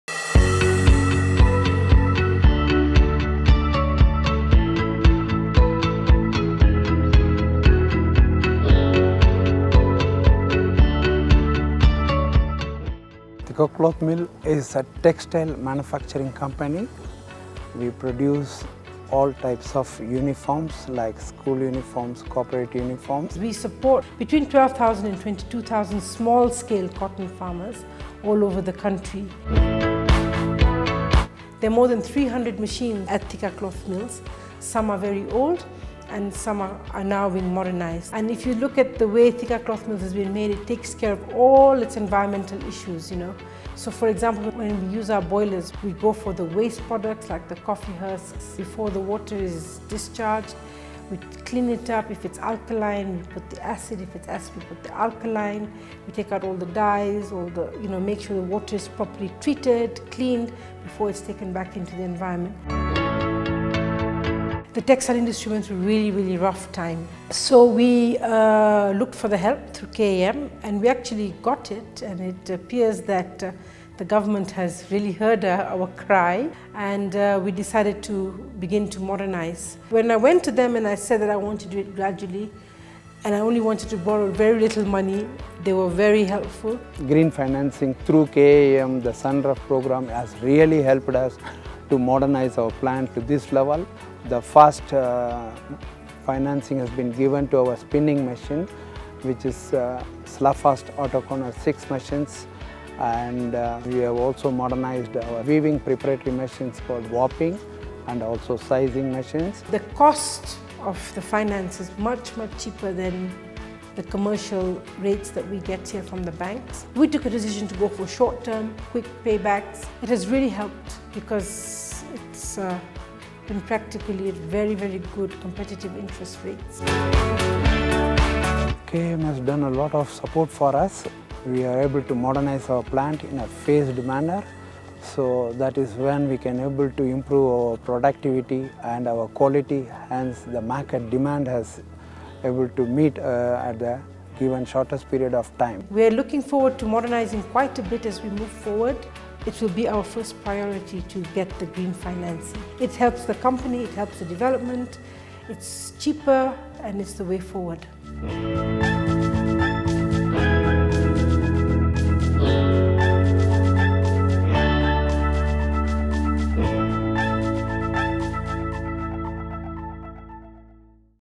Green Financing – The Path to Increasing Green Energy Production for Sustainable Economic Growth.
Through partnership with the SUNREF Programme, Kenya Association of Manufacturers (KAM) supports Kenyan industries through green financing, a tailor-made solution offered in form of concessional debt finance and free technical assistance that allows companies to acquire better quality equipment, insulate buildings, save on costs, increase competitiveness and develop green energy projects, or services and activities related to energy management and the mitigation of climate change.